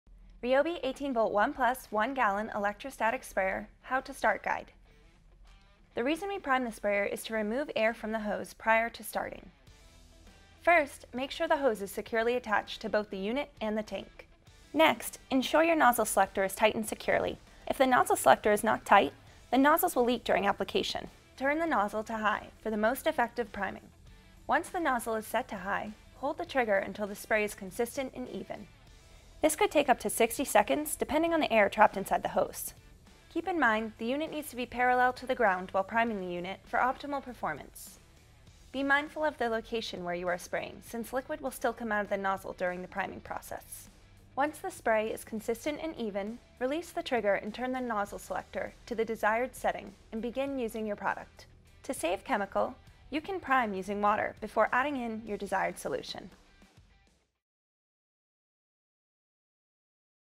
How-To: RYOBI 18V ONE+ 1 Gal. Electrostatic Sprayer Start Guide
Learn how to properly start a  RYOBI 18V ONE+ 1 Gal. Electrostatic Sprayer!